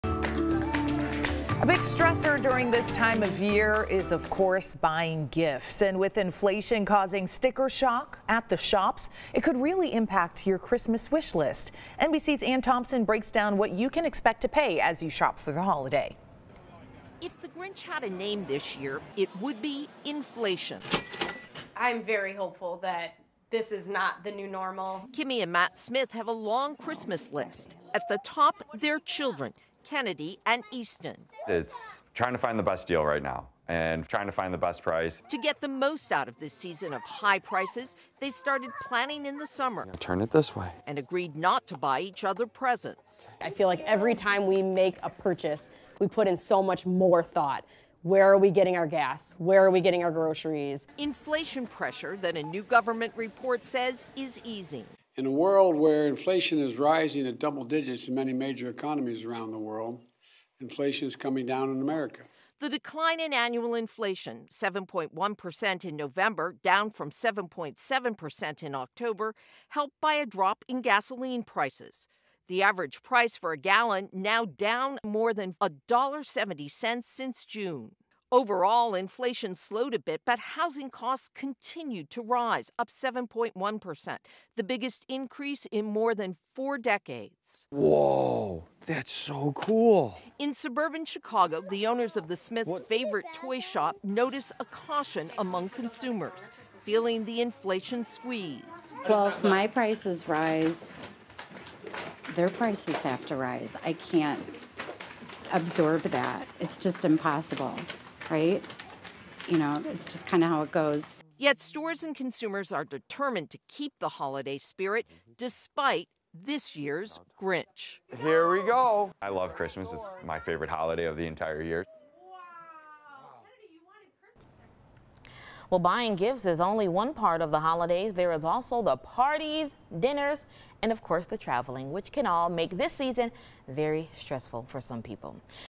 "Sticker shock" persuade customers to second guess Christmas shopping
Inflation has caused "sticker shocks" causing many customers to second-guess gifting during the holidays.